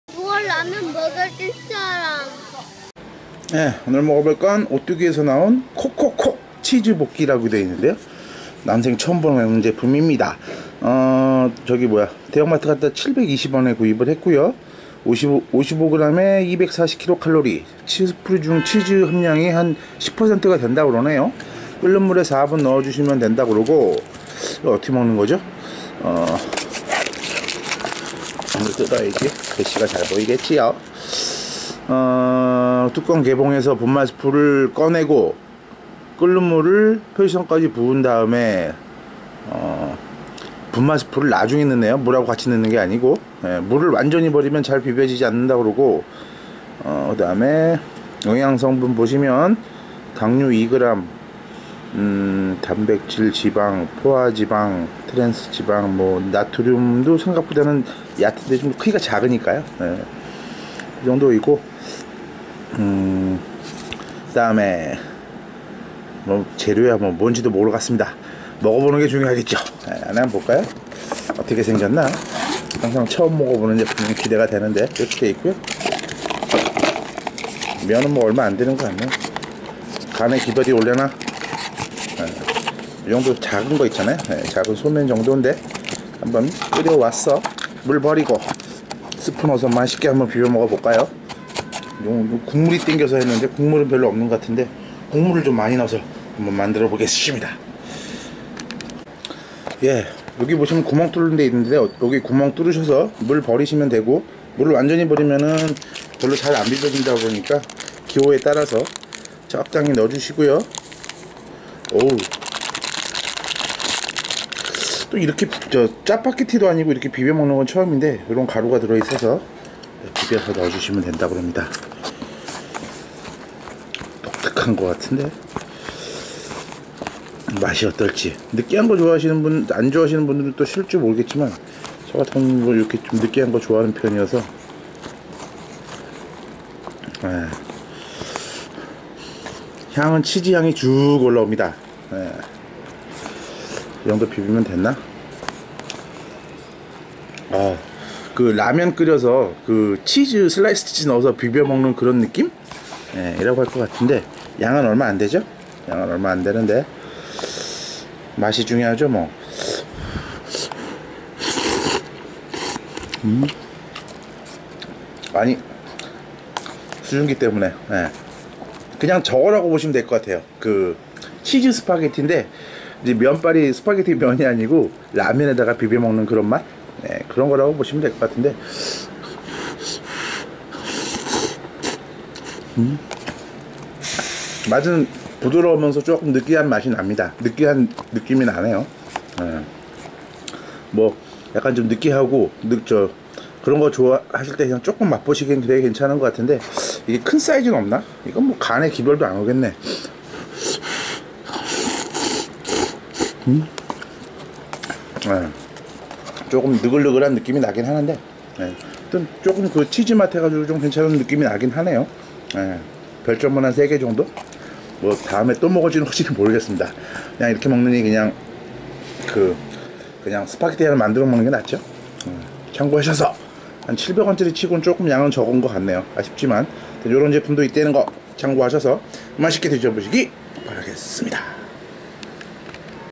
오뚜기 콕콕콕 치즈볶이 미니 컵라면, 치즈 라면 스파게티같은 제품 시식기
대형마트에서 800원정도의 가격에 구입한 제품인데, 짜파게티처럼 먹는 방식인데, 꼭 까르보나라 스파게티를 먹는듯한 느낌이 들기도 하네요~
약간 좀 느글느글한 느낌도 드는데, 양이 너무 작아서 식사 대용으로는 그렇고, 간식으로 가볍게 드시는 정도로 드셔보시면 좋을듯 합니다!

인터넷 쇼핑몰에서는 좀 더 저렴한 가격에 판매하니 참고해보세요~